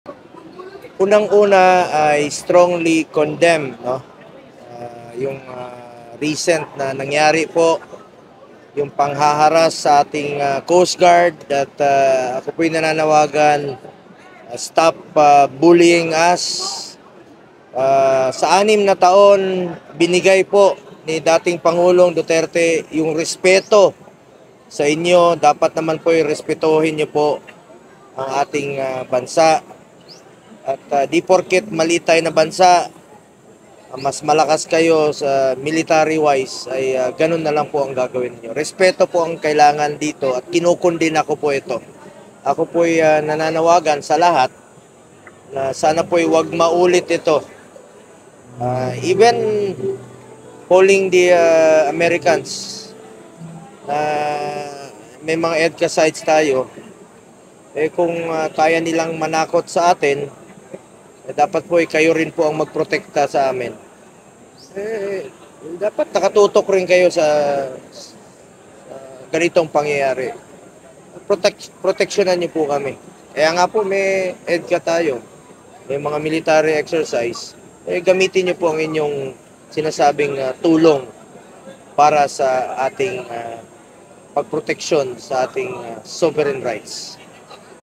'Stop bullying us': Mensahe ni Bong Go sa Tsina | ABS-CBN News
Kinondena ni Sen. Bong Go ngayong Lunes, Agosto 7, ang paggamit ng Chinese Coast Guard ng water cannons sa Philippine Coast Guard vessels habang nagsasagawa ang mga Pilipino ng resupply mission sa Ayungin Shoal noong Agosto 5.

Ayon sa senador, dapat suklian ng Tsina ng respeto ang Pilipinas, lalo’t naging maganda ang relasyon ng dalawang bansa sa ilalim ng administrasyon ni dating Pangulong Rodrigo Duterte.

Nanawagan din ang senador sa Estados Unidos na proteksyunan ang mga PCG sa West Philippine Sea laban sa aniya’y “harassment” ng CCG.